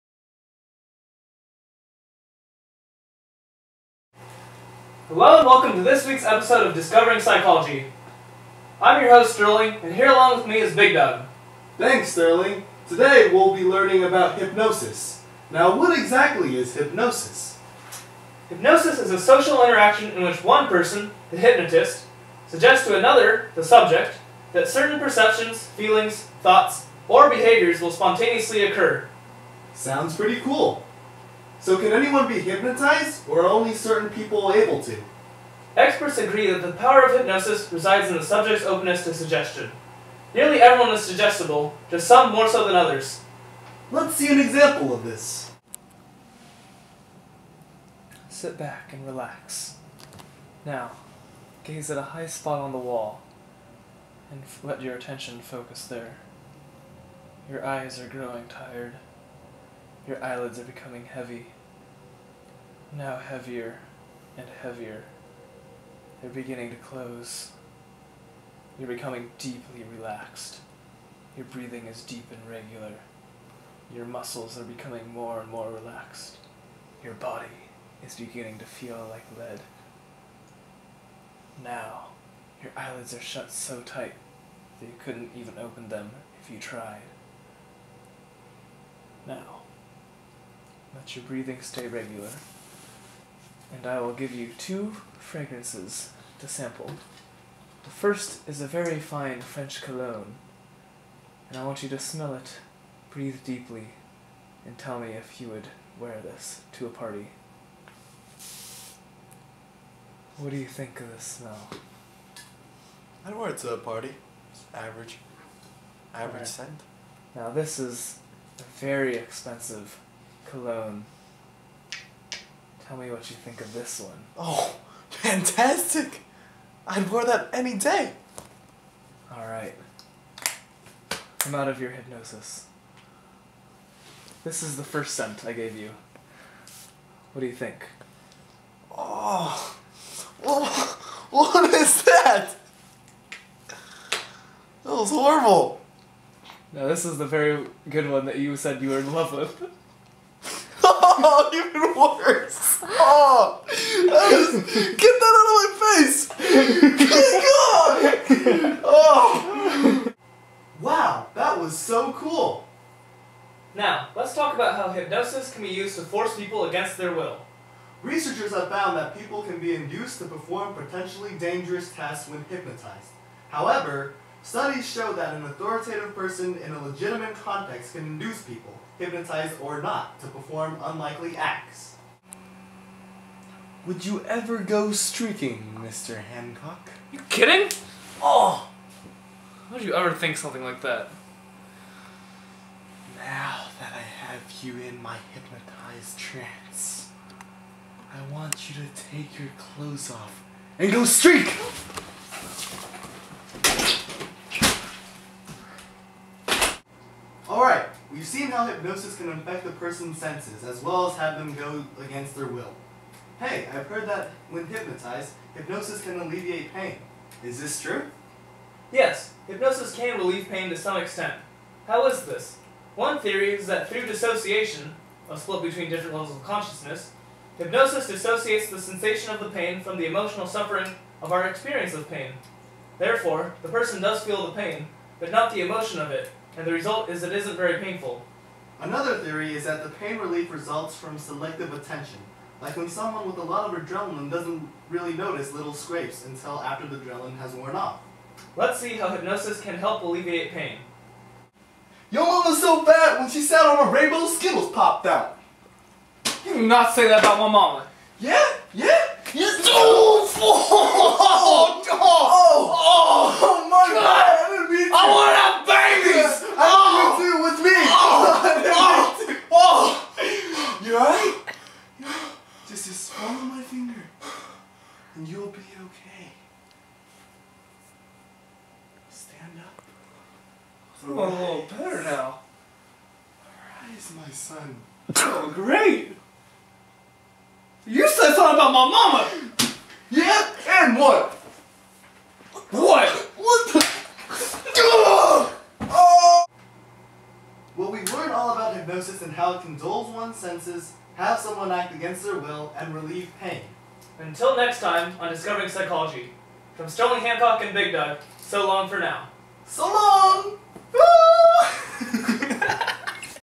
Discovering Psychology - Hypnosis
AP Psych Final Project

made in the style of Dr. Philip Zimbardo's documentary "Discovering Psychology"